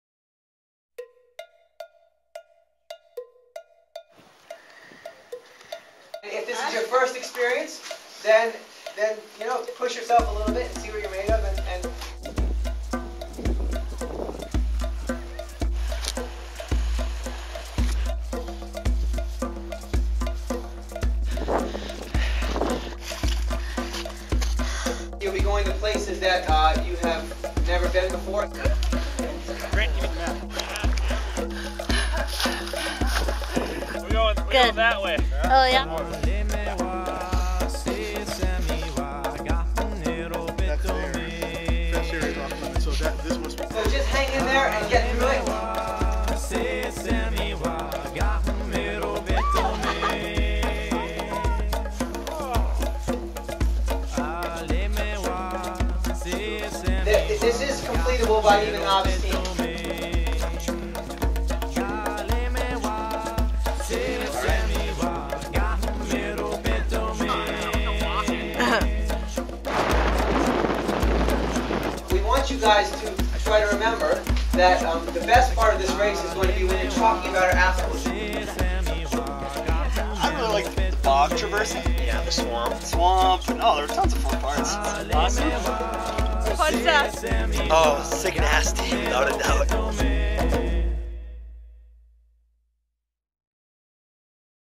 Extreme North Dakota Spring Primer Adventure Race 2010 (movie 1)
here's the first of 4 videos jason hopes to put together from footage of END-SPAR 2010.  If you were there i hope it reminds you of what an animal you are!  if you weren't there, i hope it makes you realize you need to be next time (Sept 11th baby!)